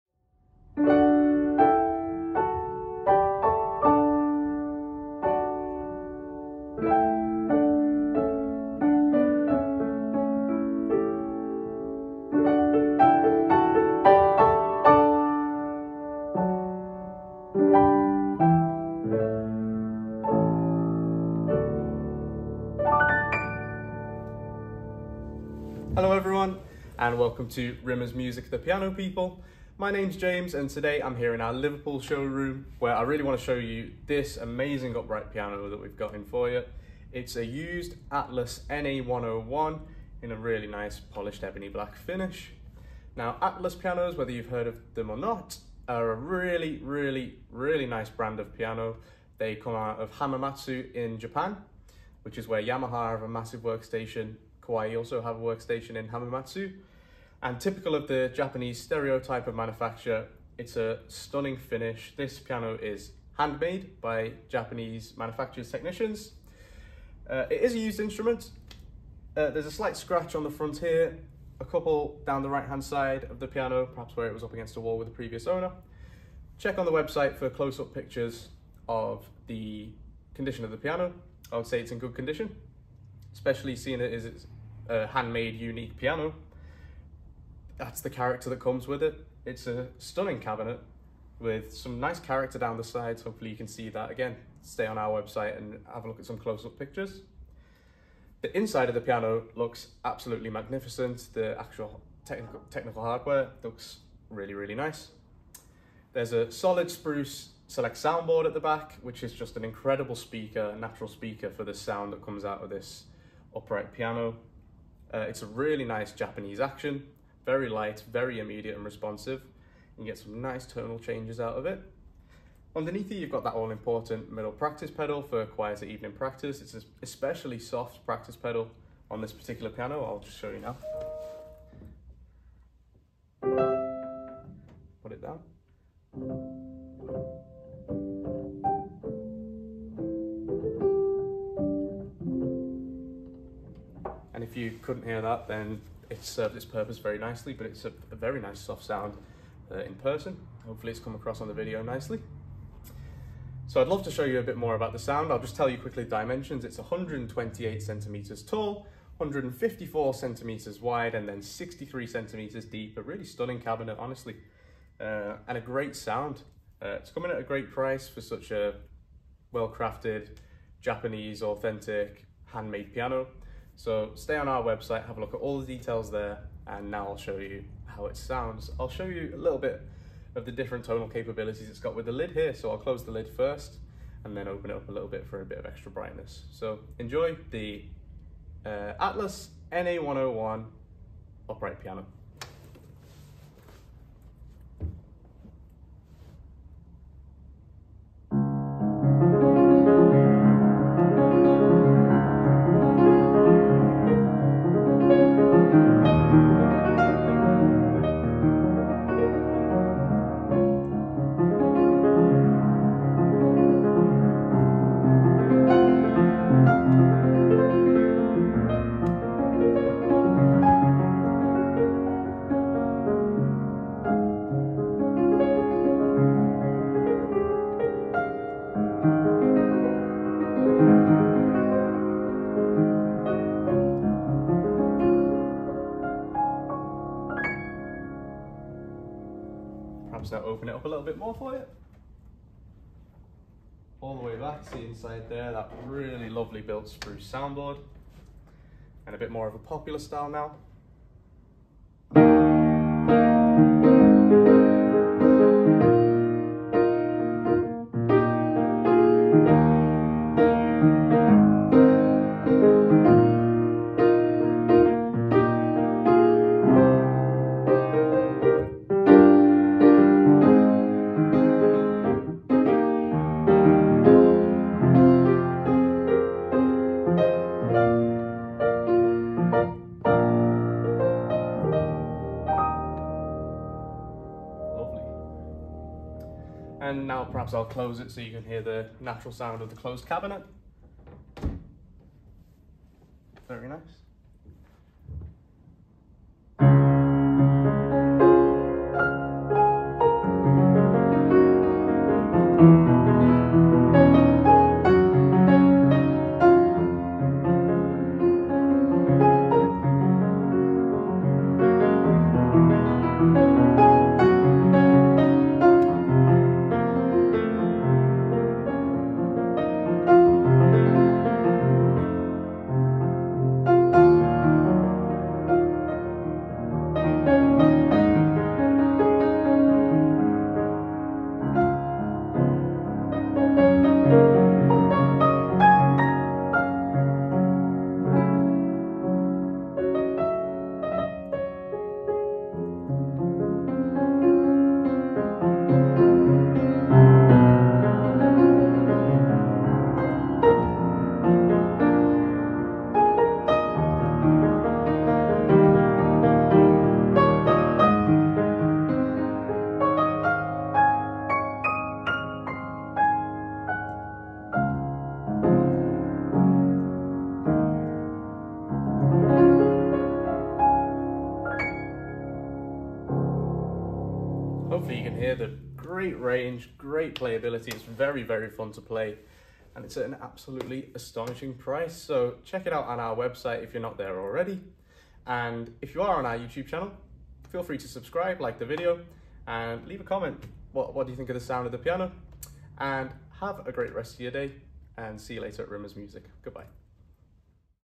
Used Atlas NA101 Upright Piano Demonstration & Review - Rimmers Music Liverpool
Here we have James McMullen showing you why you need to buy this brilliant Atlas NA101B Upright Piano at Rimmers Music Liverpool.

Atlas pianos were hand made in Hamamatsu Japan, the same city where Yamaha and Kawai pianos are made. This piano has been designed and handmade to a very high build quality. It has a wonderful tone, with a very deep and resonant bass that progresses through to a bright and singing treble. The tone can be attributed to the quality of master crafted soundboard.  The Atlas action is immediate and responsive, very accurate, it allows the pianist to play to the softest of volumes and then to the loudest with ease of control.
This Atlas Upright Piano is fitted with middle pedal that when engaged activates a practice rail so quiet time playing is easily attainable without compromising the touch quality and control of the instrument, this is an ideal feature to have when practice is desirable with others in close proximity.
James McMullen - Rimmers Music 'The Piano People'